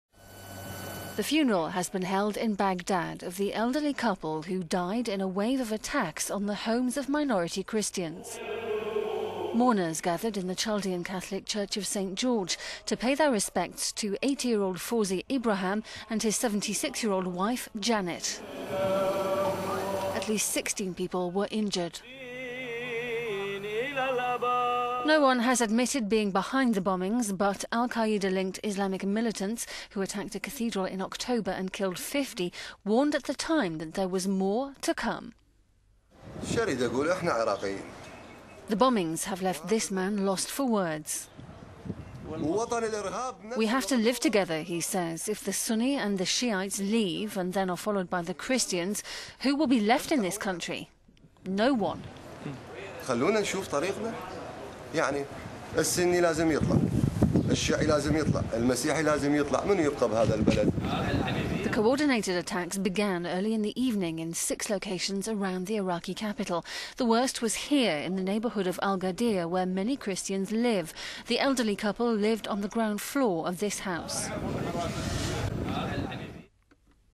Bagdad Christian attacks: elderly victims buried
The funeral has been held in Baghdad of the elderly couple who died in wave of attacks on the homes of minority Christians.

Mourners gathered in the Chaldean Catholic Church of St George to pay their respects to 80-year-old Fawzi Ibrahim and his 76-year-old wife Janet.

At least 16 other people were injured.

No one has admitted being behind the bombings.

But al-Qaeda-linked Islamic militants, who attacked a cathedral in October and killed 50, warned at the time of more to come.